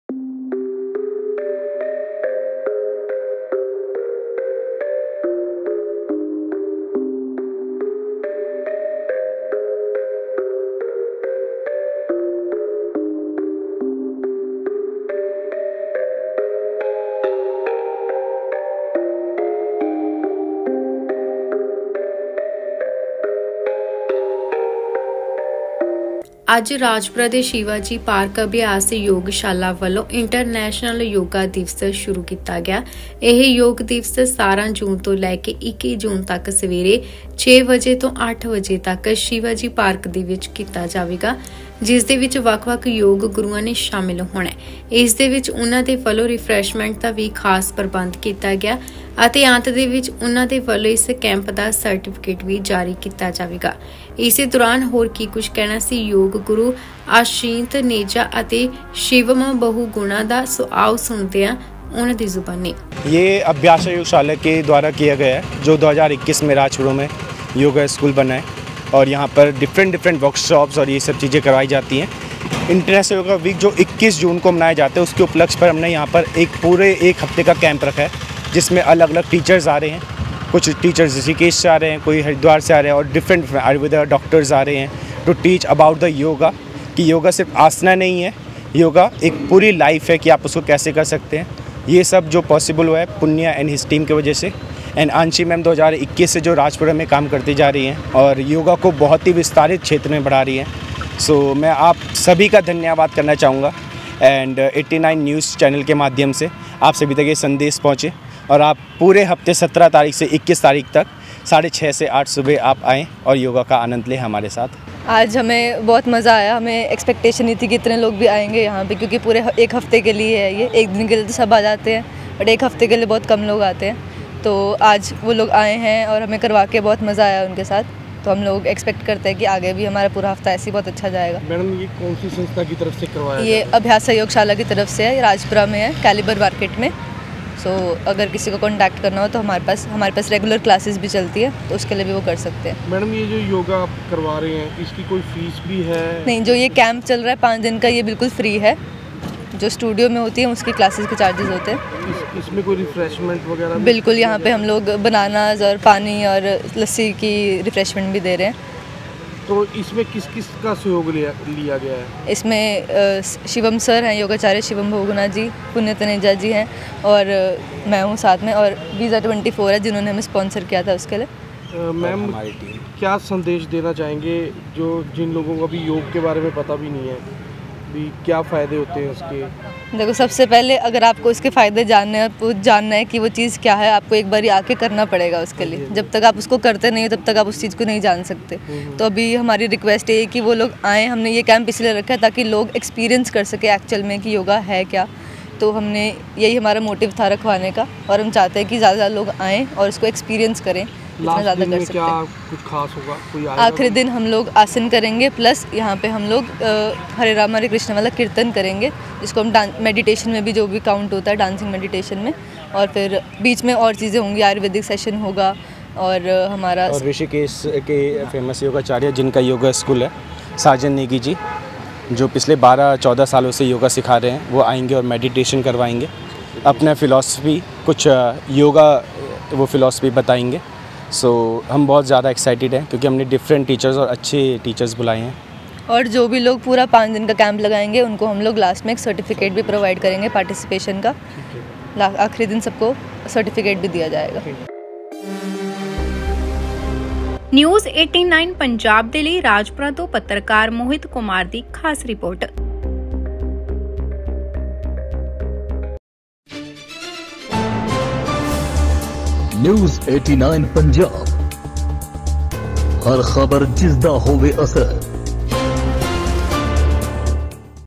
👉 ਅਭਿਆਸ ਯੋਗਸ਼ਾਲਾ ਵੱਲੋਂ ਸ਼ਿਵਾਜੀ ਪਾਰਕ ਵਿਚ ਇੰਟਰਨੈਸ਼ਨਲ ਯੋਗਾ ਦਿਵਸ ਕੀਤਾ ਗਿਆ ਸ਼ੁਰੂ
👉ਯੋਗ ਦਿਵਸ 17 ਤੋਂ ਲੈ ਕੇ 21 ਜੂਨ ਤੱਕ ਸਵੇਰੇ 6ੇ ਤੋਂ 8 ਵਜੇ ਤੱਕ ਸ਼ਿਵਾਜੀ  ਪਾਰਕ ਵਿੱਚ ਕੀਤਾ ਜਾਵੇਗਾ